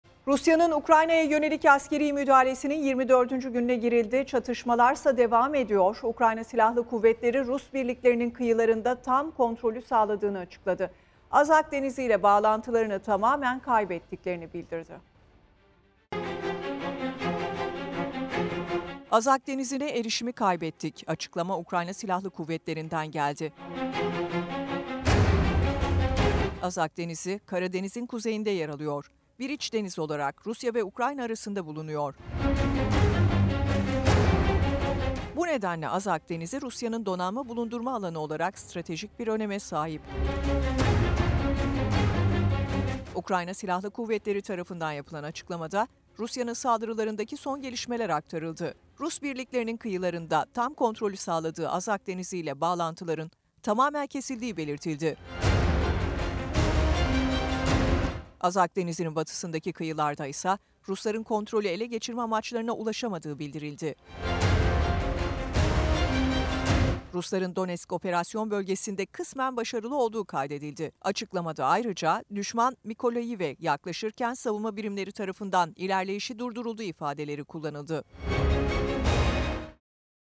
İlerleyişini Sürdüren Rusya, Ukrayna'nın Azak Denizi'ne Olan Bağlantısını Kesti
Ukrayna Silahlı Kuvvetleri, Rus birliklerinin kıyılarında tam kontrolünü sağladığı Azak Denizi ile bağlantılarını tamamen kaybettiklerini açıkladı.